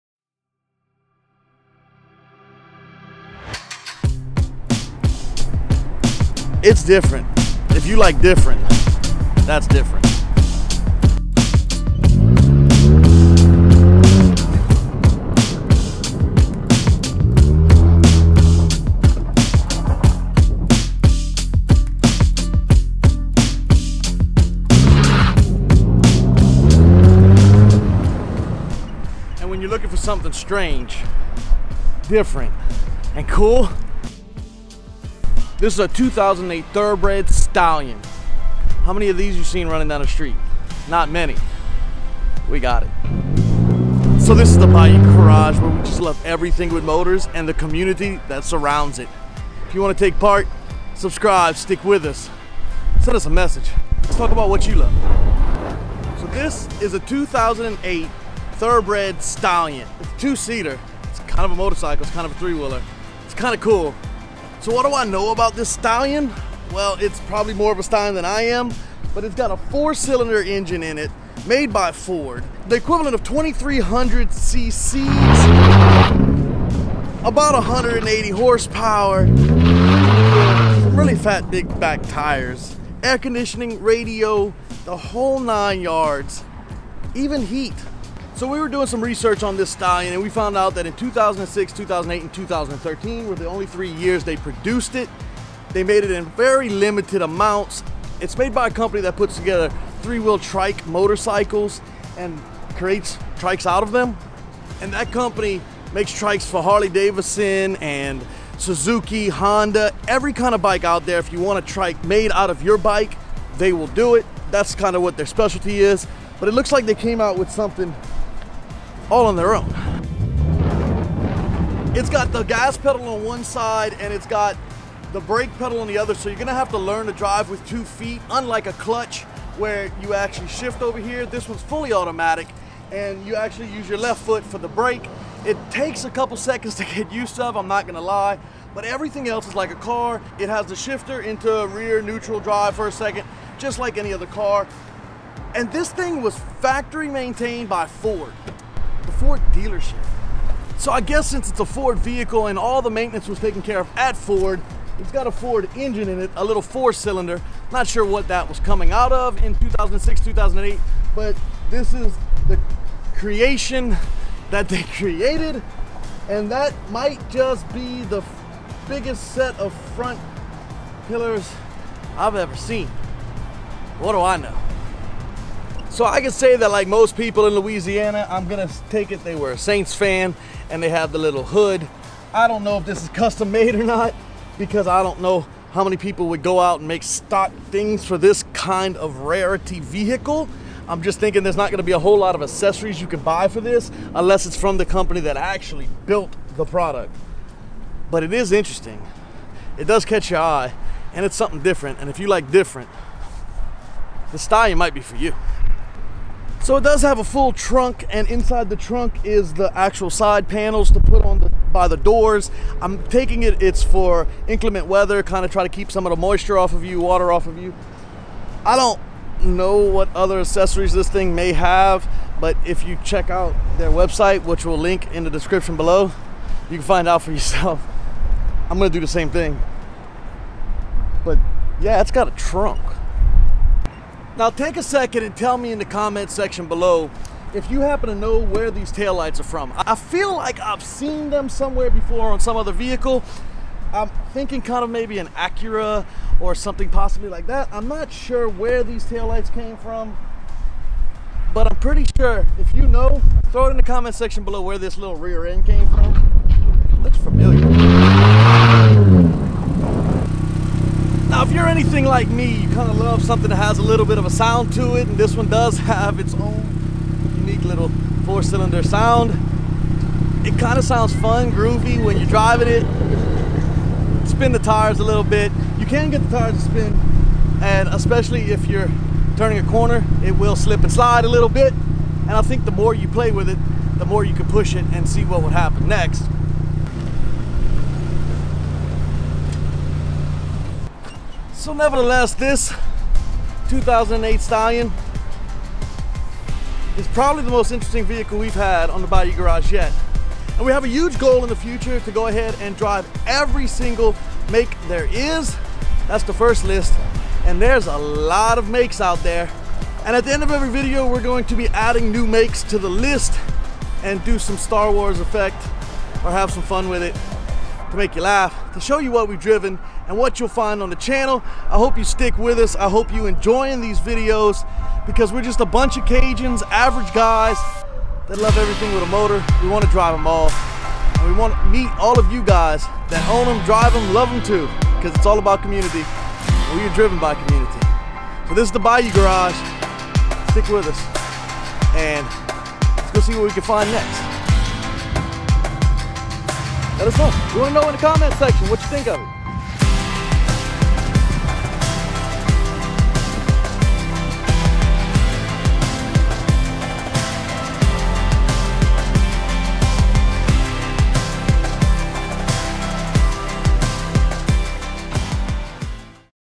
Wild MotorCycle Trike - 2008 Thoroughbred Stallion
Wow, this is one wild Motorcycle Trike.  This 2008 Thoroughbred Stallion is an amazingly fun two-seater that you have to see to believe.  We at the Bayou Garage love anything with a motor and this is no different!  Yes is has AC and Heat, Music and so much more!  It was maintained by Ford Motor Company.